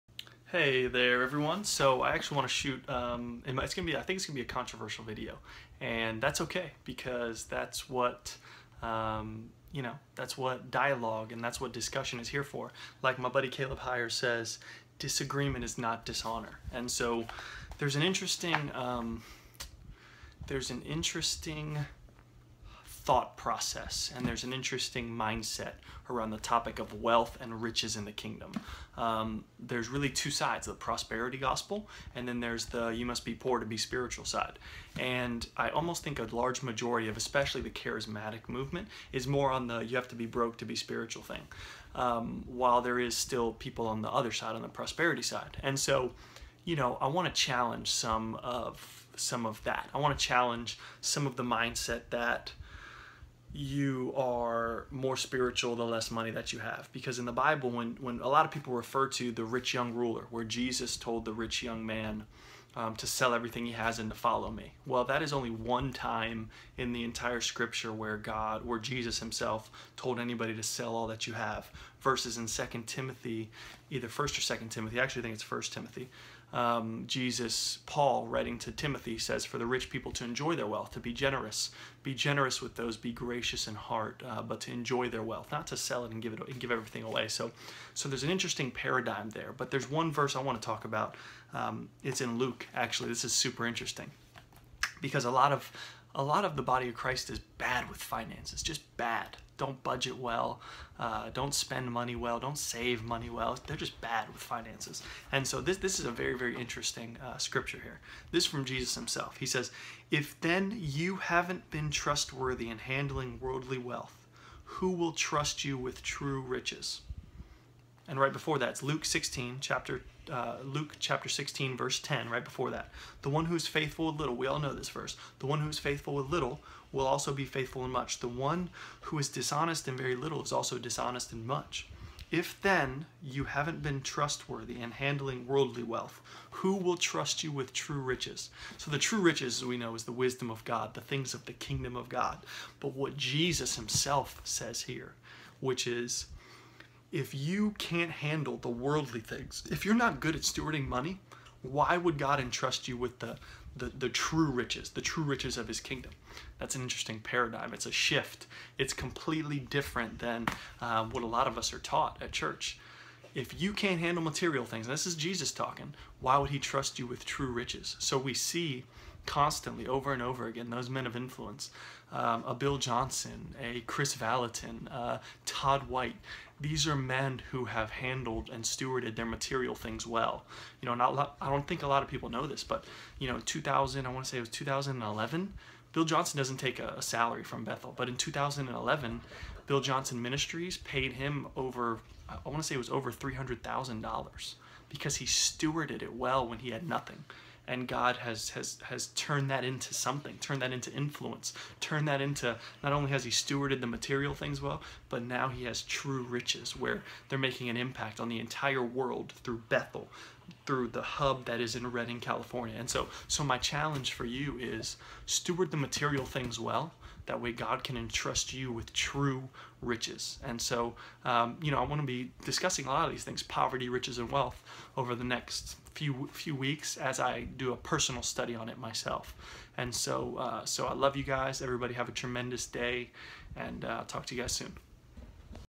Can you be trusted with true riches?
In Luke 16 : 10-11 Jesus says “"He who is faithful in a very little thing is faithful also in much; and he who is unrighteous in a very little thing is unrighteous also in much. Therefore if you have not been faithful in the use of unrighteous wealth, who will entrust the true riches to you?”
‭‭Luke‬ ‭16:10-11‬ ‭NASB‬‬

Are you stewarding the material things in your life well? Stewarding material wealth will lead to the true riches of the kingdom of God.